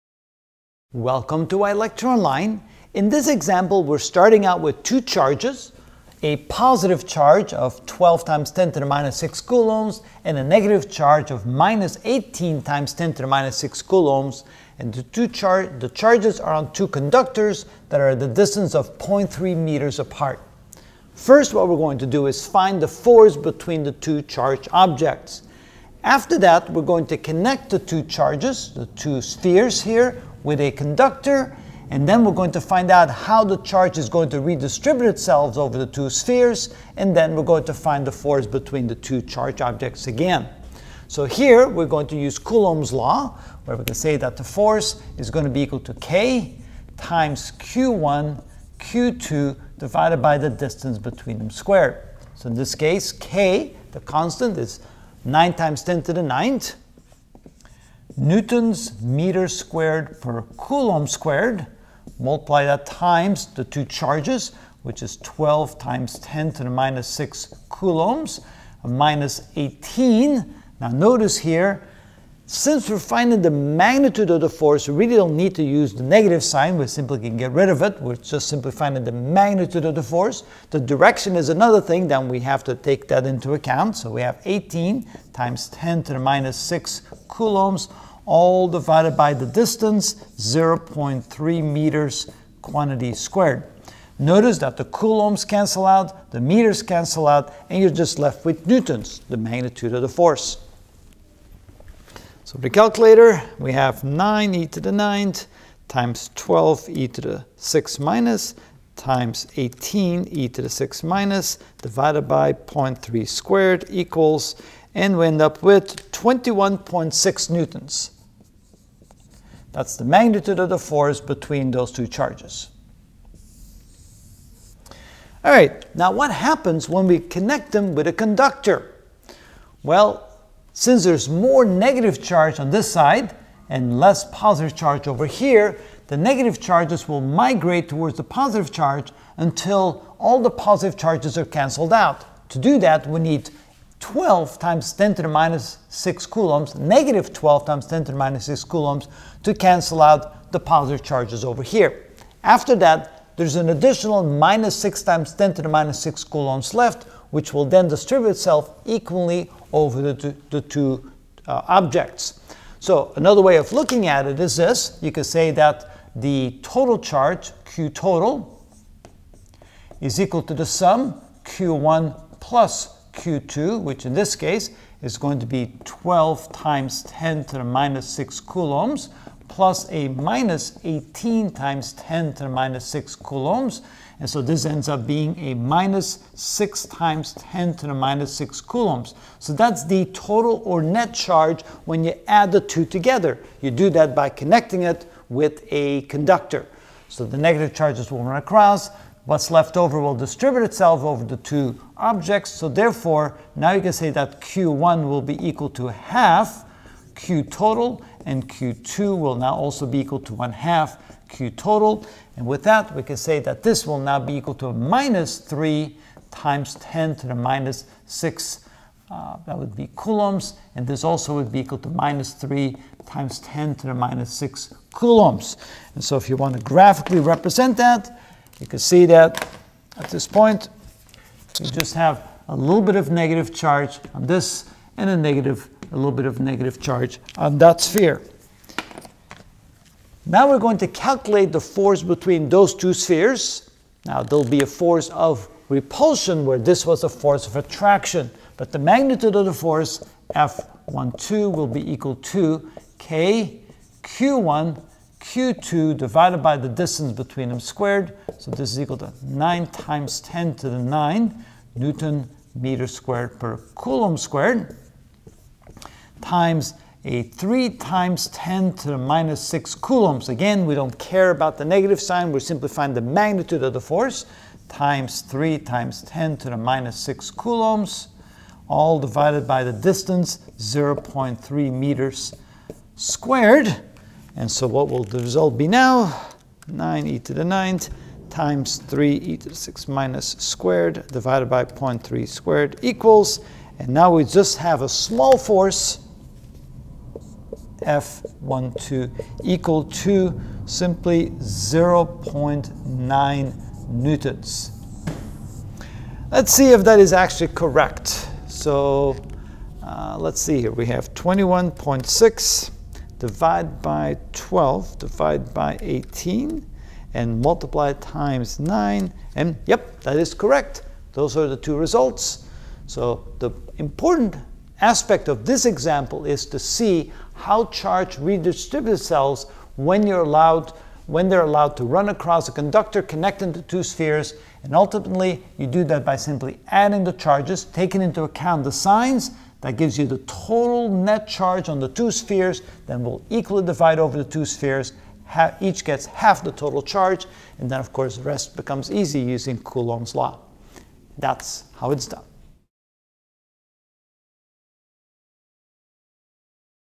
Physics - E&M: Ch 35.1 Coulumb's Law Explained (22 of 28) Conducting Sphere Connect
In this video I will find the force, F=?, of 2 positively and negatively charge conductors q1 and q2 a distance d=0.3m apart.  Next I will find q1=? and q2=? when the 2 charges are connected with a conductor.